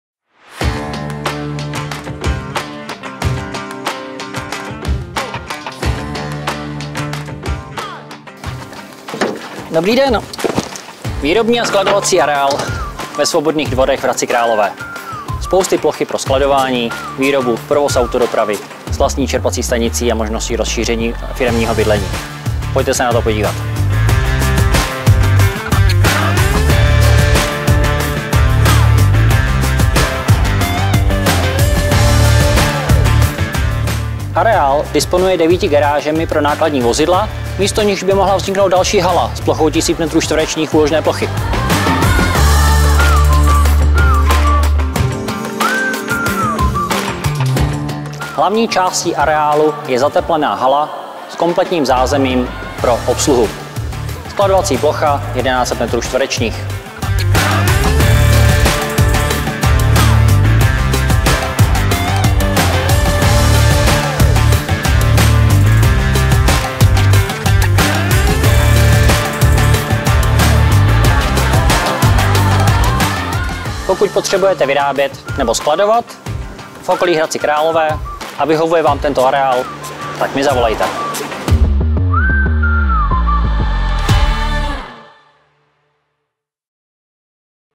Video 292833 k nabídce ID30165
Nabízíme k prodeji skladovací a výrobní areál na okraji Hradce Králové, ve Svobodných Dvorech. Areál se sestává z výrobní haly se schválenou motorárnou pro opravy nákladních vozidel, průmyslové haly s jeřábovou dráhou, zateplené haly pro skladování nebo lehkou výrobu, garáží, administrativní budovy se sociálním zařízením a služebním bytem, neveřejné čerpací stanice, nevyužívané budovy vrátnice a rozsáhlé odstavné a parkovací plochy pro kamiony.
K areálu vede vlastní příjezdová cesta pro nákladní vozidla s dálkovým ovládáním vrat. Z ulice Dvorská je samostatný vjezd pro osobní vozidla a přístup pro pěší. Areál je napojený na inženýrské sítě (voda, kanalizace, elektřina, plyn) a navíc disponuje vlastní studnou.

Zpevněné a parkovací plochy činí 5548 m2
Celková výměra všech pozemků 8110 m2

Prodej je možný jako nemovitost nebo až 100% spoluvlastnického podílu společnosti, jejíž jedinou činností je vlastnictví a pronájem nabízeného areálu. Nenabízíme rozprodej po částech, prodej je možný pouze jako celek.
Výrobní haly jsou dlouhodobě výhodně pronajaty. Hlavní hala se skladovací plochou 1200 m2  je v současné době volná a připravená k dalšímu pronájmu nebo pro využití k vlastní činnosti nový majitelem. V areálu je možné na místě garáží pro nákladní vozidla vybudovat další výrobní nebo skladovací halu s plochou 1100 m2.

Součástí prodeje je projekt s platným stavebním povolením na přístavbu vzorkové prodejny, bytů a ubytovny.